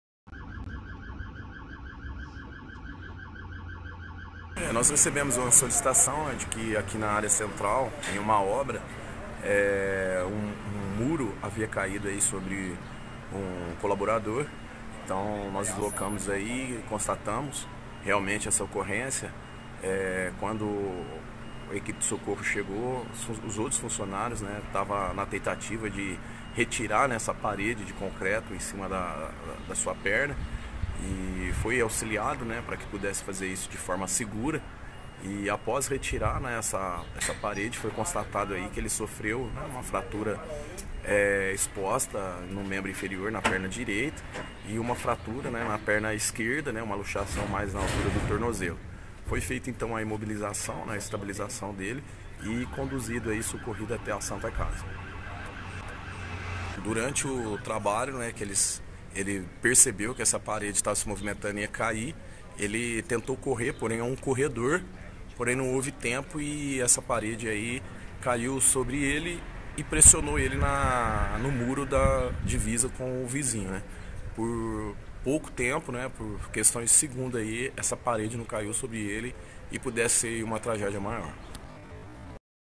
Trabalhador sofre ferimentos graves após desabamento no centro de Franca
Um trabalhador ficou gravemente ferido na tarde desta terça-feira, 3, após uma parede que estava sendo demolida desabar sobre ele. O acidente ocorreu na Rua do Comércio, na região central de Franca.

Segundo o Sargento Diógenes Toledo, do Corpo de Bombeiros, o trabalhador tentou correr ao perceber que a parede estava cedendo, mas acabou sendo atingido.

Outros trabalhadores ajudaram a vítima até a chegada do Corpo de Bombeiros que retirou o trabalhador dos escombros e após ser resgatado, o homem foi socorrido para a Santa Casa com fraturas nas pernas.

A Polícia Militar foi acionada para registrar a ocorrência. As causas do acidente serão investigadas.